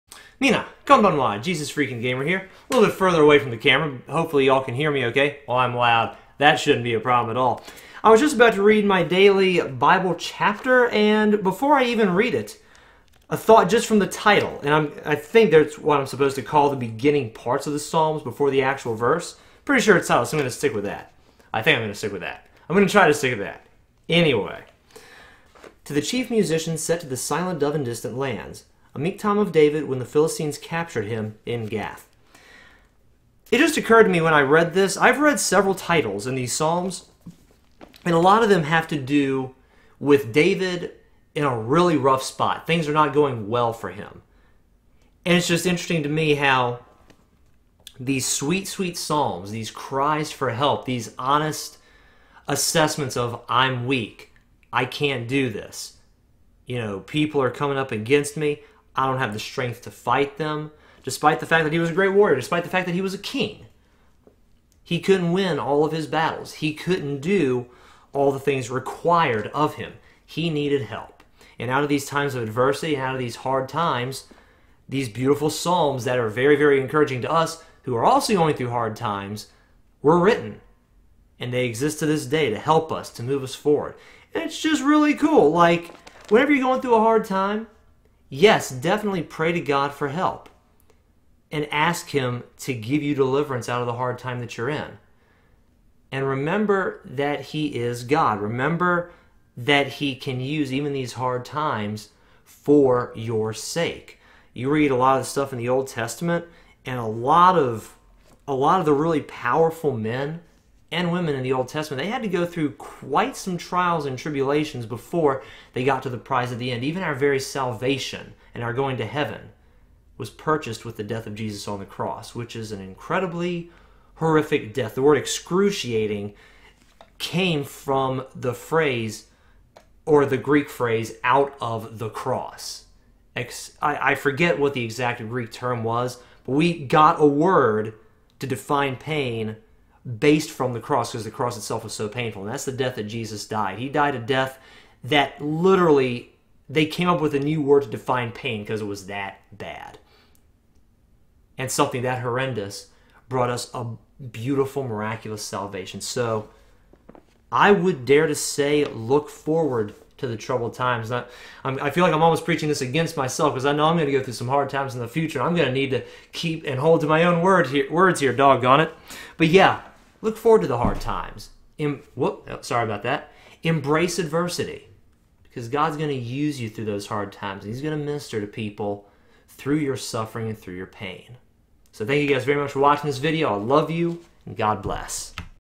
BEAUTIFUL PRAISE BIRTHED OUT OF TERRIBLE PAIN
David got a lot of inspiration from the hard times he went through. We should use him as our example.